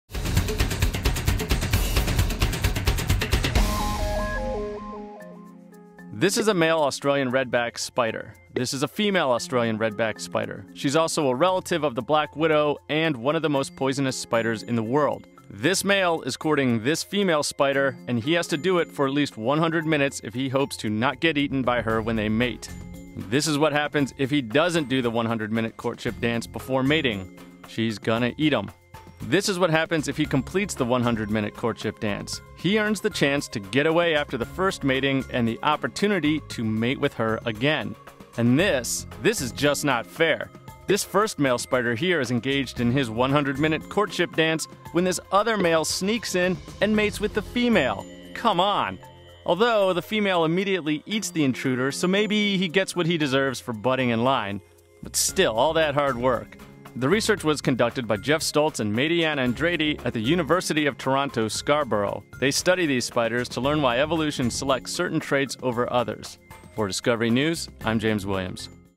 Spider Sex Dangerous For Some
The longer a male Australian redback spider woos the female, the better chance she won't eat him the first time they mate. James Williams explores this tangled web of poisonous spider sex.